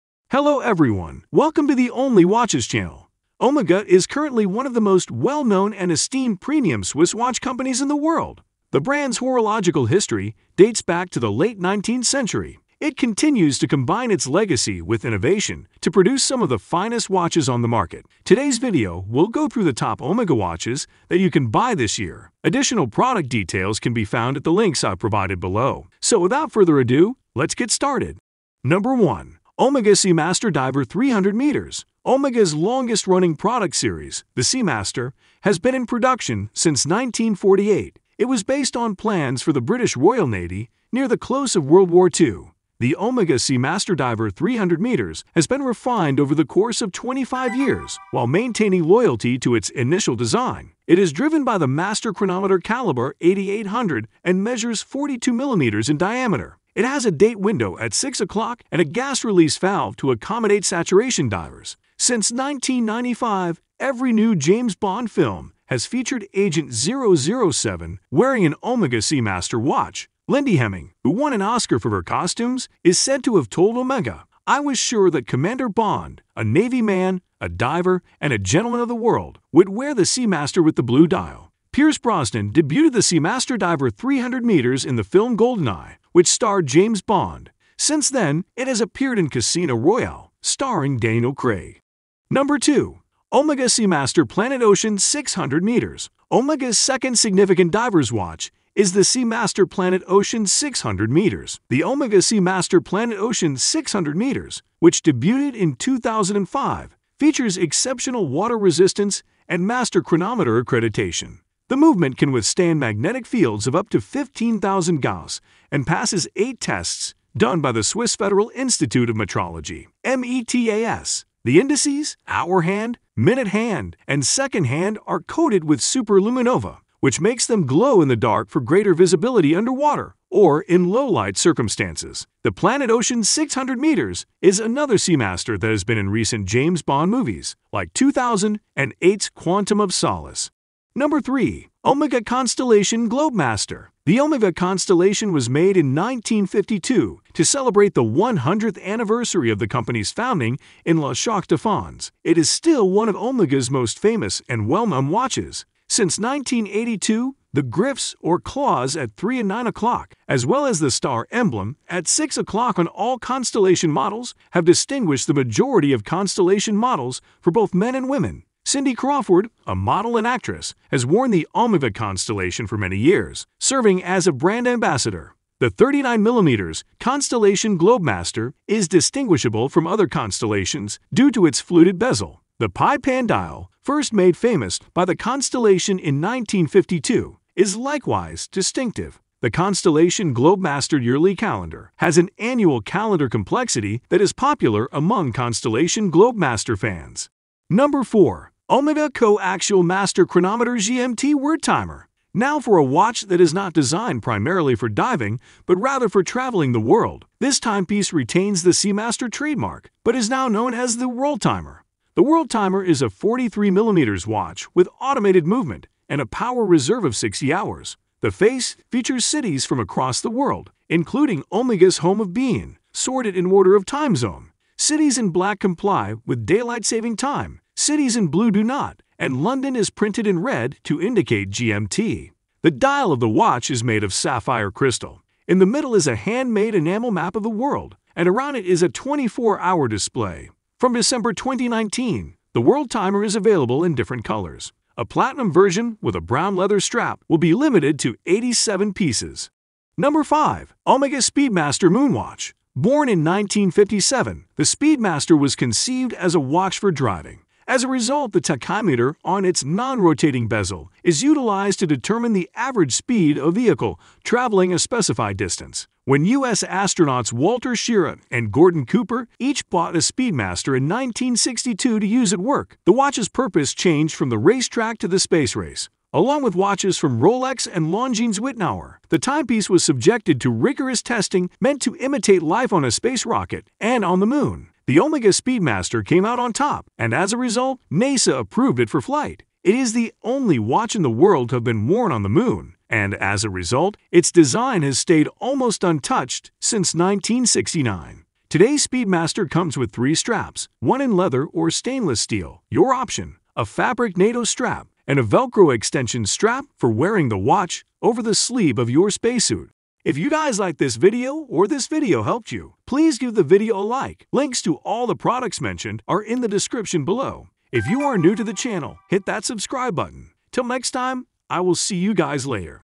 TOP 5 BEST OMEGA WATCHES 2023
⭐1-Omega Seamaster Diver 300M
⭐ Amazon International
⭐2-Omega Seamaster Planet Ocean 600M
⭐ Amazon International
⭐3-Omega Constellation Globemaster
⭐ Amazon International
⭐4-Omega Seamaster Worldtimer
⭐ Amazon International
⭐5-Omega speedmaster moonwatch
⭐ Amazon International